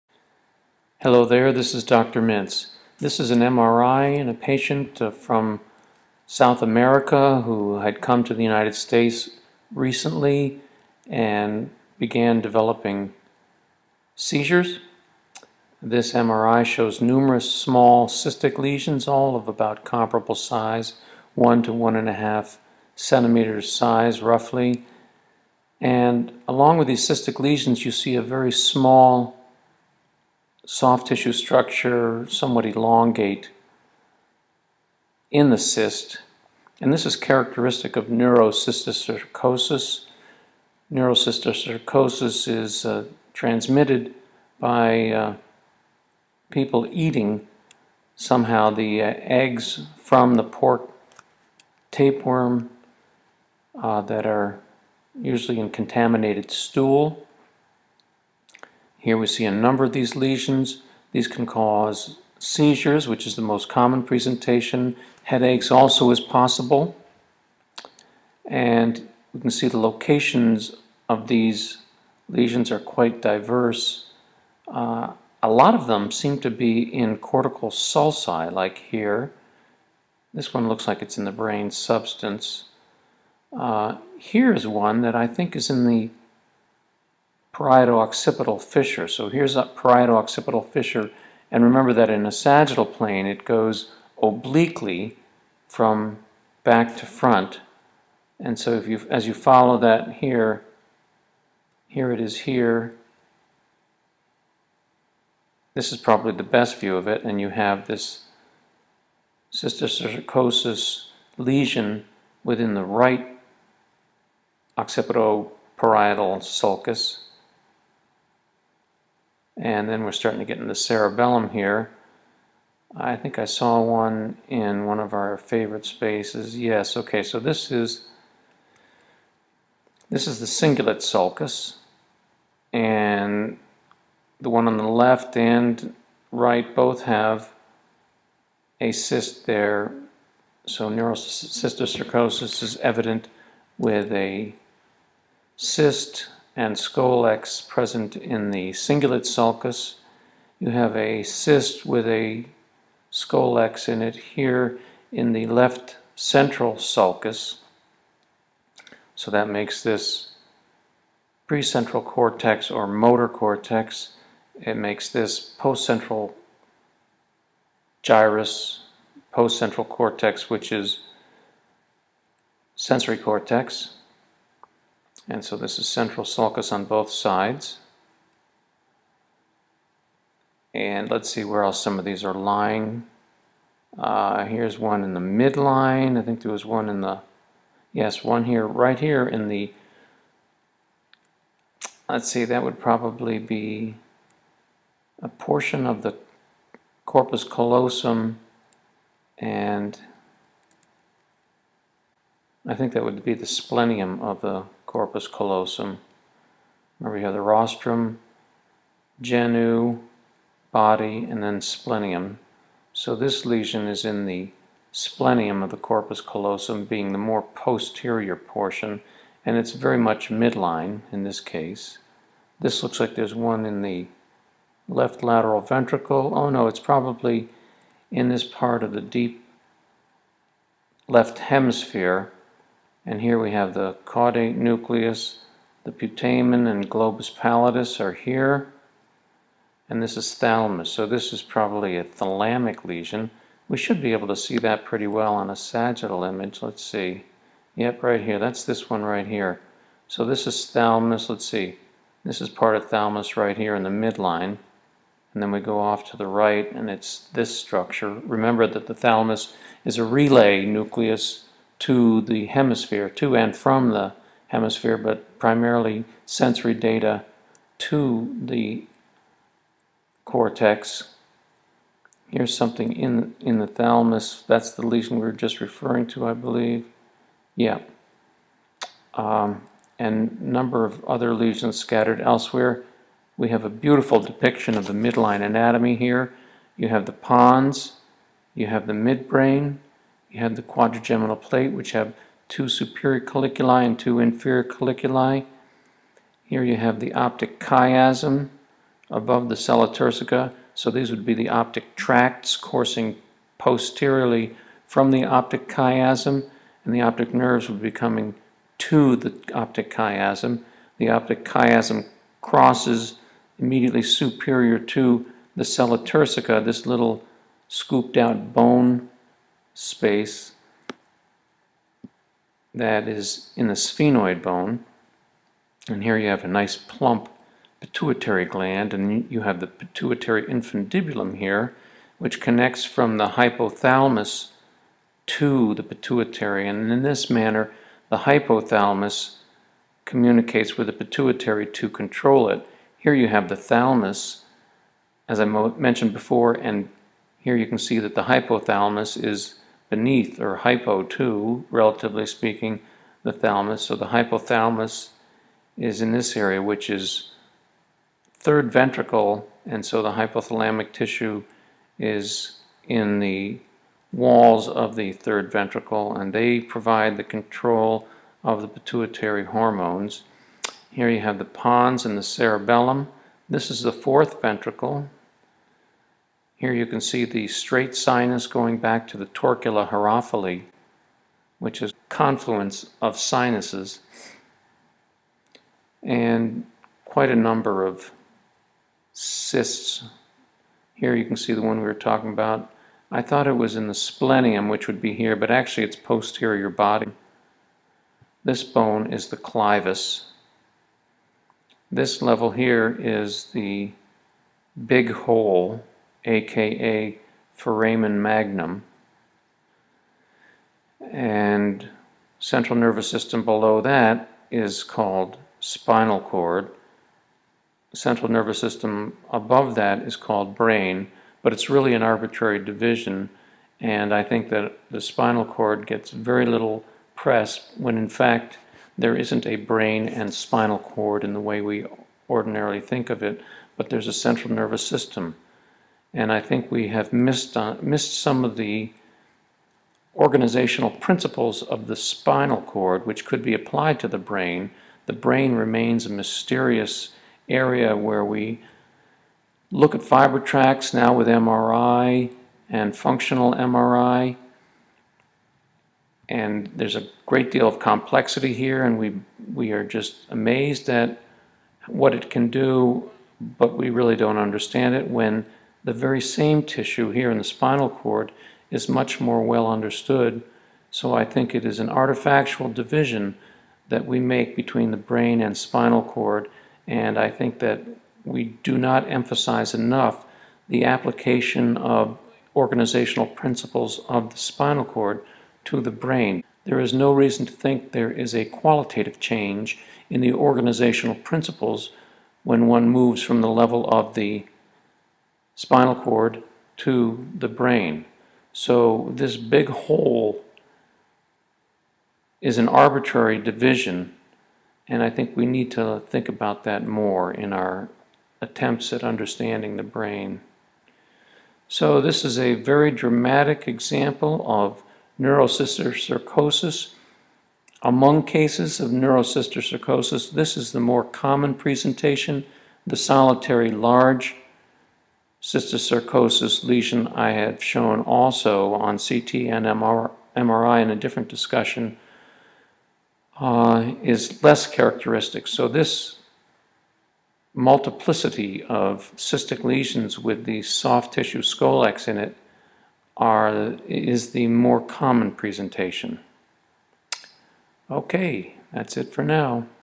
Cysticercosis (multiple) of Brain MRI DISCUSSION
This is a discussion of an MRI of the brain in a 30yoM with Numerous cysticercosis lesions of the brain.